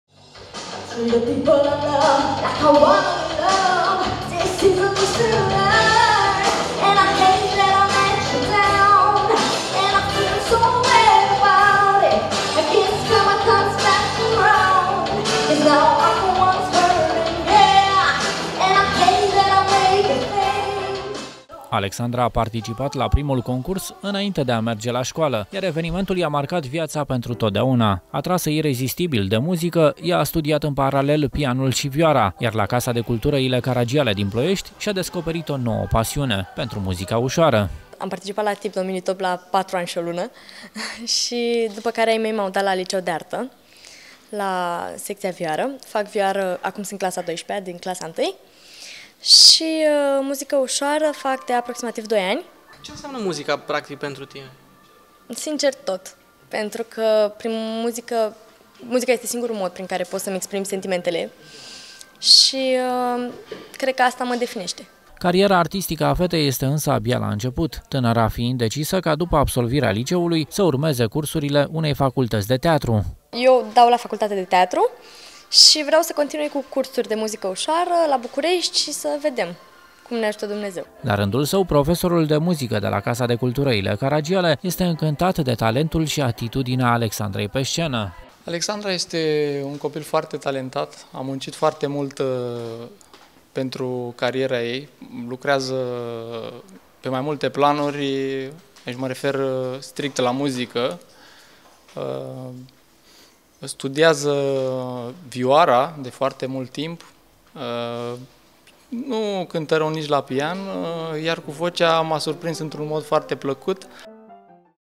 MUZICA E VIAŢA EI!
Un simplu concurs de interpretare pentru copii a ajuns sa-i schimbe viata unei tinere din Ploiesti care acum,  la varsta de 18 ani,  canta la doua ,iar  cu vocea sa reuseste de fiecare data sa incante publicul si juriile festivalurilor la care participa.